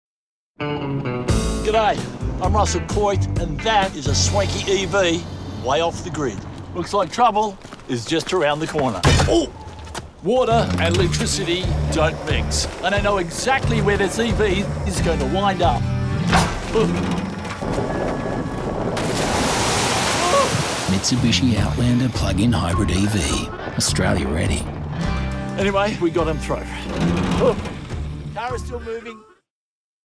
Outlander Plug-in Hybrid EV | Water & Electricity
Russell Coight senses trouble around the corner, but no obstacle is too big or small with the Outlander Plug-in Hybrid EV!